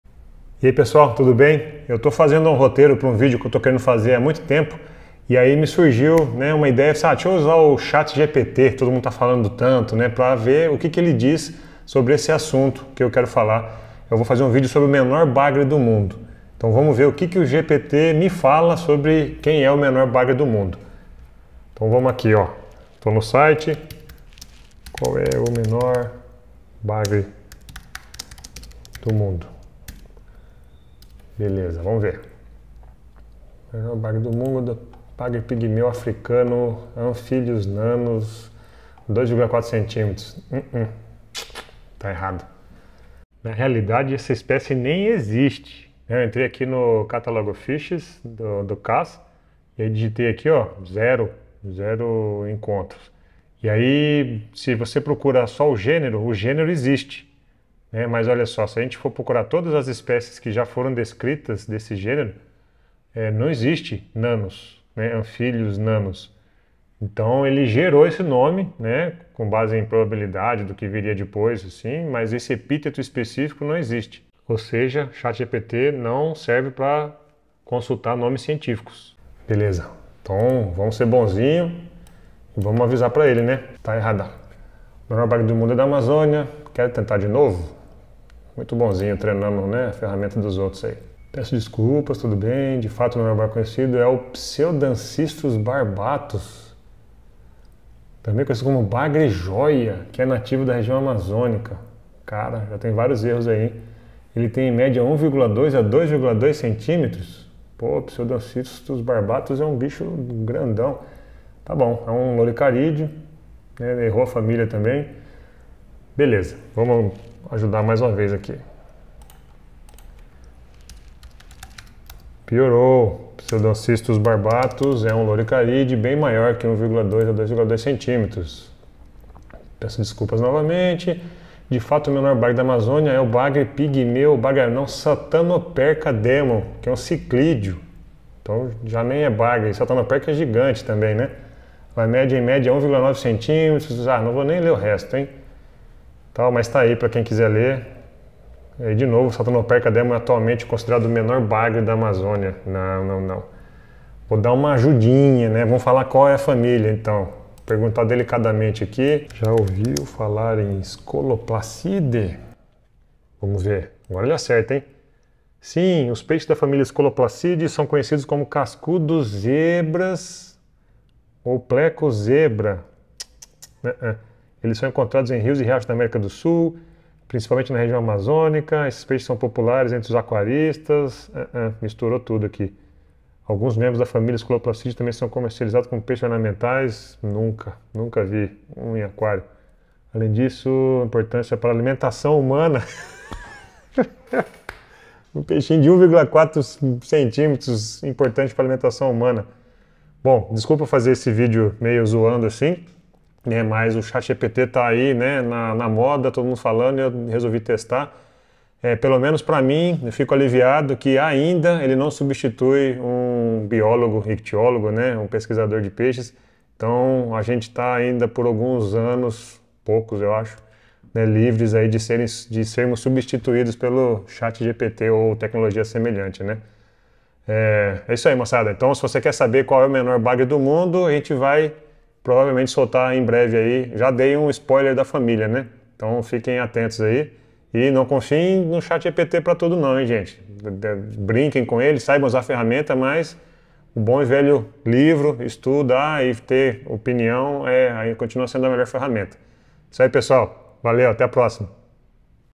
ChatGPT SABE NADA de peixe!
Fiz uma busca despretensiosa no ChatGPT para ver se ele acertava uma pergunta "simples" sobre peixes. Qual o menor bagre do mundo?
As respostas foram surpreendentes...de tão ruins!
É claro que esta tecnologia vai avançar muito rapidamente, mas por enquanto, só fala besteira com relação à taxonomia e diversidade de peixes.
A questão mais importante: o texto que ele gera segue todas as regras gramaticais e faz sentido semântico. Se alguém não conhece do assunto, pode até achar que está certo. E esse é o principal PERIGO desta ferramenta.